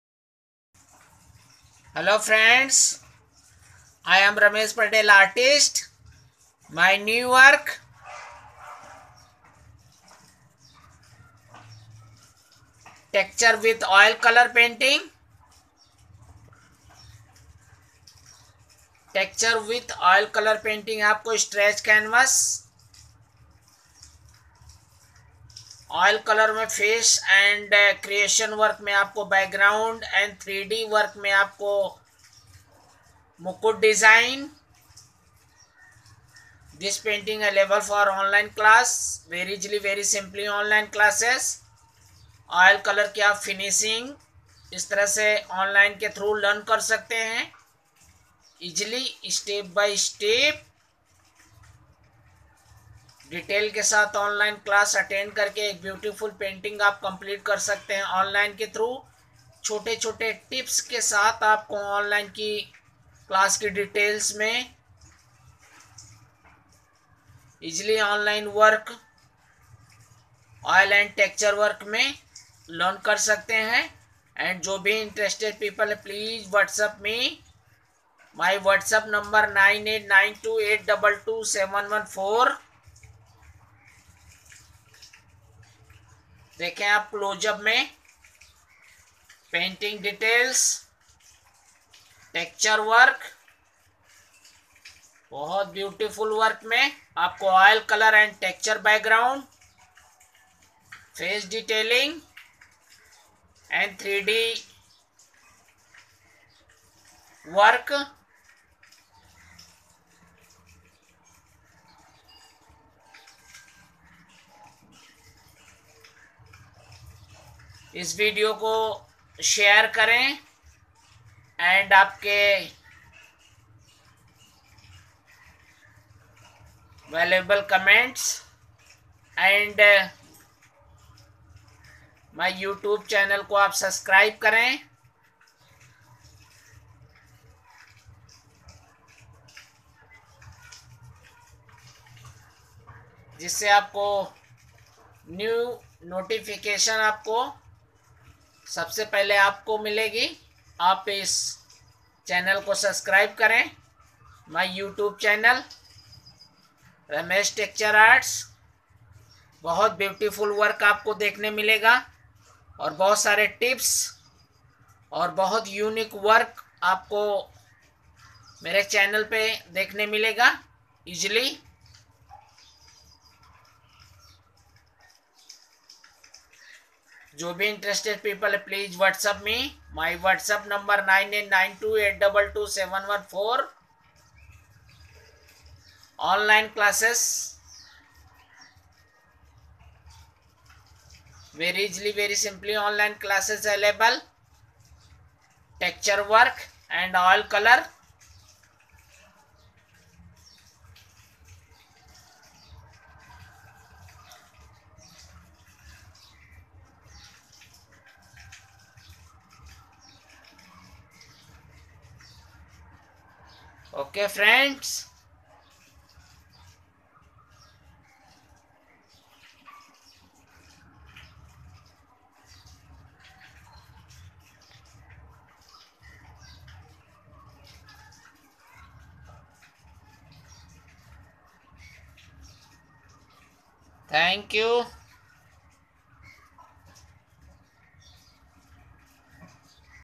Lord Trimurti Painting
hello friends
I'm Ramesh Patel artist
texture work
oil and acrylic on canvas
available for online classes